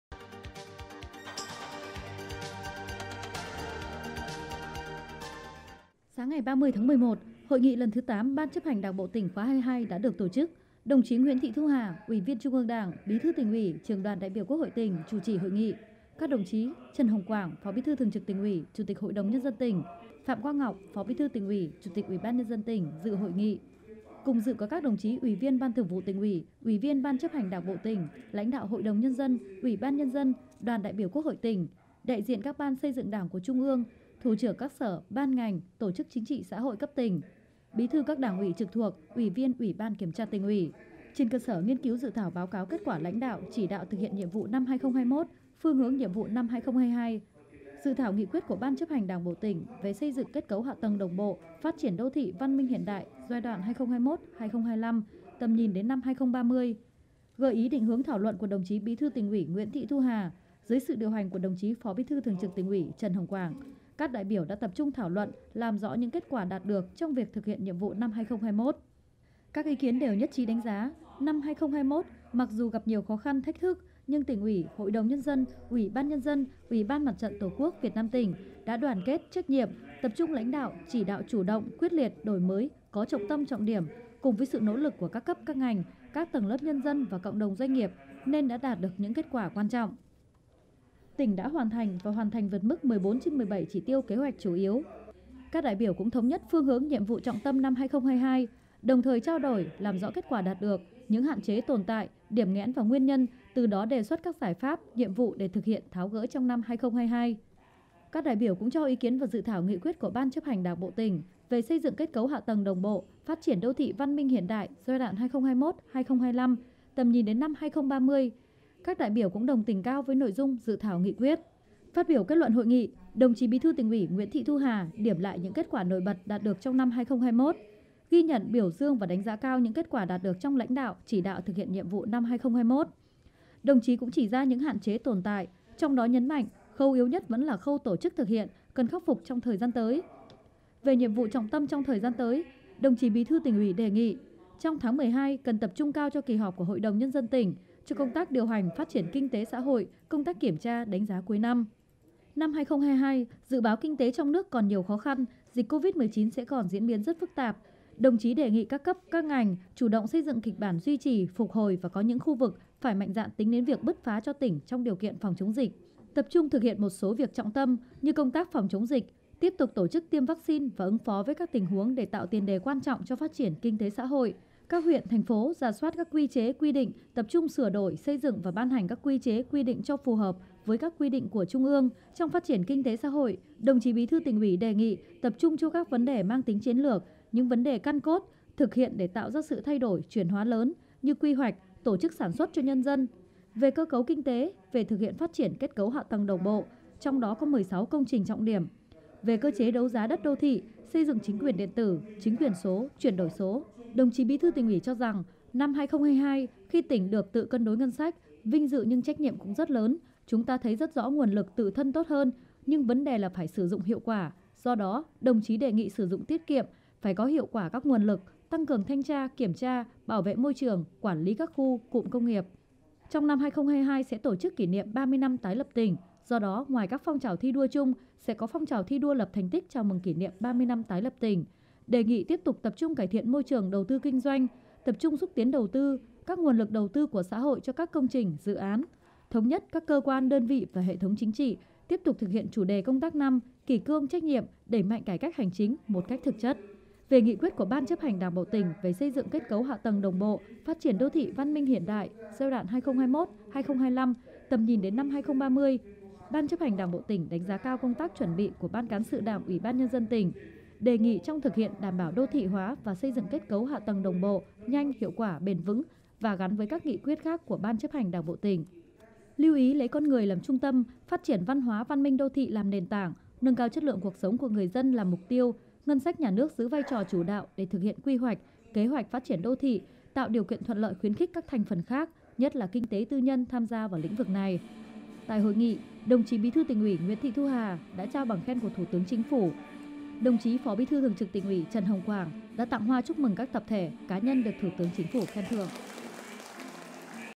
Hội nghị lần thứ tám, Ban Chấp hành Đảng bộ tỉnh khóa XXII
Sáng 30/11, Hội nghị lần thứ tám, Ban Chấp hành Đảng bộ tỉnh khóa XXII đã được tổ chức. Đồng chí Nguyễn Thị Thu Hà, UVT.Ư Đảng, Bí thư Tỉnh ủy, Trưởng đoàn ĐBQH tỉnh chủ trì hội nghị.